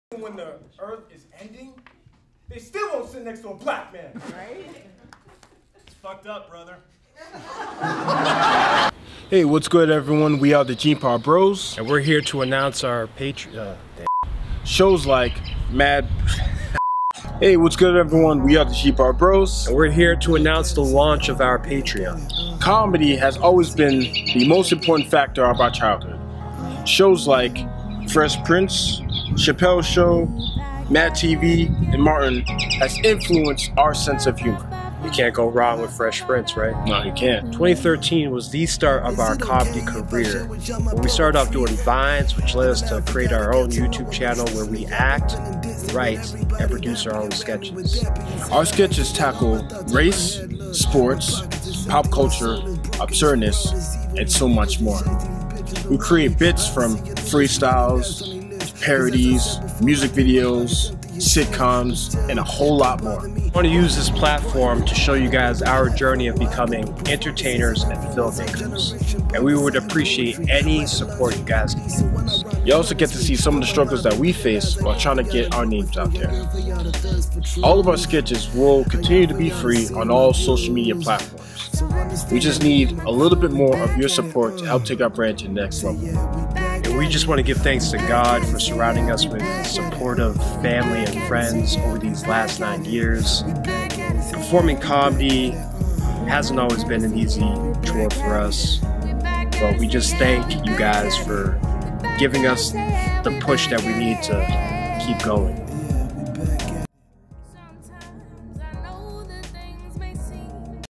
JEANPAULBROS: Launching our Official Patreon
Whats Good!
 We're the JP BROS and we are a sketch duo and aspiring filmmakers. We are officially Launching our Patreon to allow us to do what we love full-time. If you enjoy our content, please support us by becoming a patron. we would appreciate all the support you can give us. You can also follow us on all social's.
IG&Twitter: JEANPAULBROS
FB: JEANPAULBROS Production Series